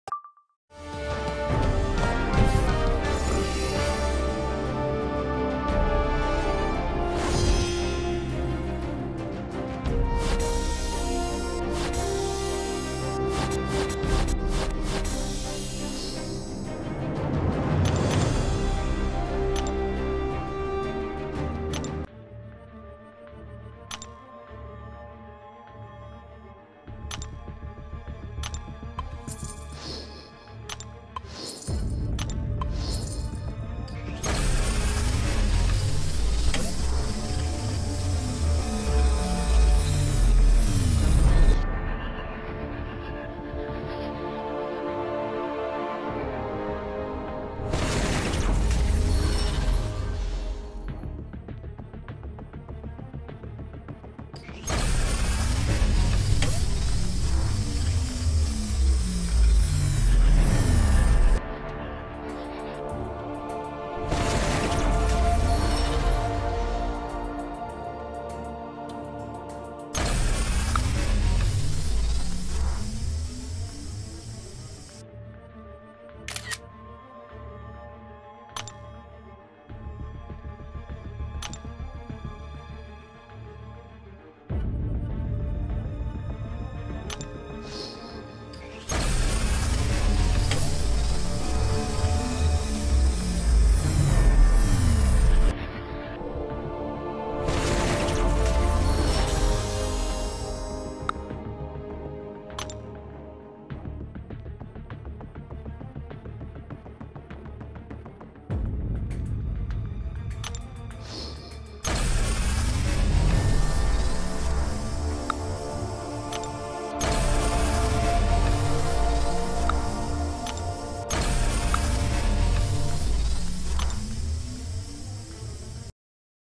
Act 8.1 initial completion rewards opening
Act 8.1 1st run rewards opening 3 featured crystals opening